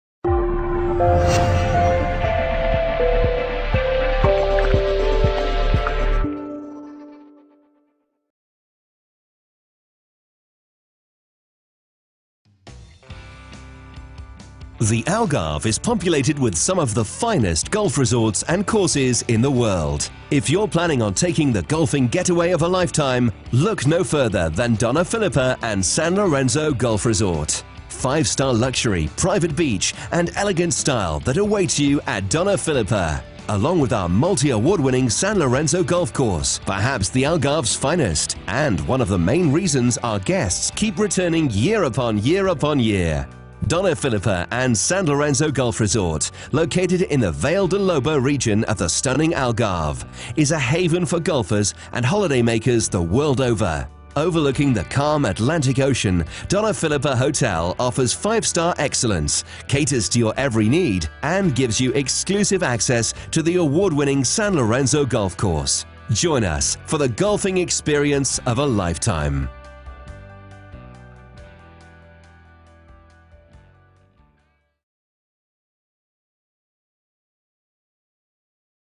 Dona Filipa & San Lorenzo - Luxury Golf Resort Portugal
Part of the Luxury Golf Break.com Classic Collection and one of Europe's premier golf holiday destinations, guests will enjoy five star luxury at the renowned Dona Filipa resort, and the challenge of San Lorenzo, consistently ranked among Portugal's very best.